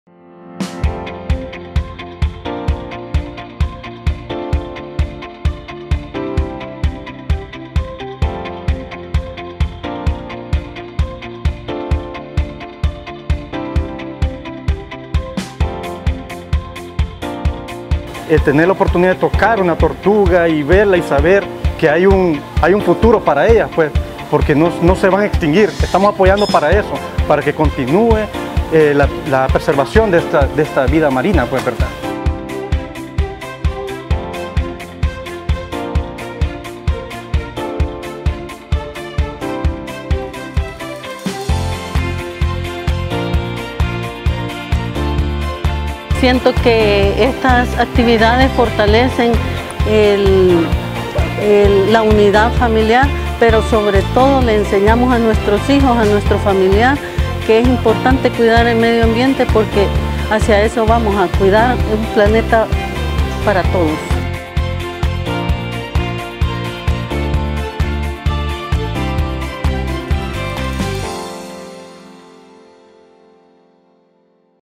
AES El Salvador apoya la preservación de la tortuga marina
Por noveno año consecutivo, más de 100 voluntarios de AES El Salvador junto a sus familiares, participaron en la liberación de neonatos de tortuga marina, para contribuir con su preservación y fomentar una cultura de responsabilidad medioambiental en las nuevas generaciones.